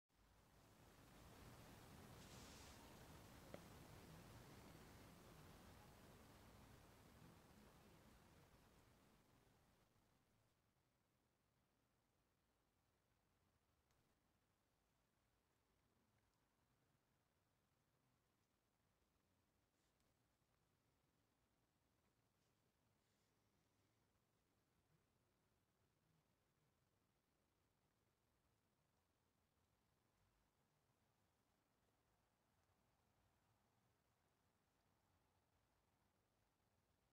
Hungry Fox
Miss Fox, 2 September 2022.

Miss Fox has known me for her whole life. It has taken two years for her to trust me this much. She was the runt of her litter (2020) and is naturally small, even for a fox. She may look thin but they all do at this time of year without their winter coats.

Miss Fox became a mother for the first time in spring 2022.

Miss Fox had mange earlier this year but I was able to successfully treat her with ivermectin.

She used to be known as Fox Cub#3.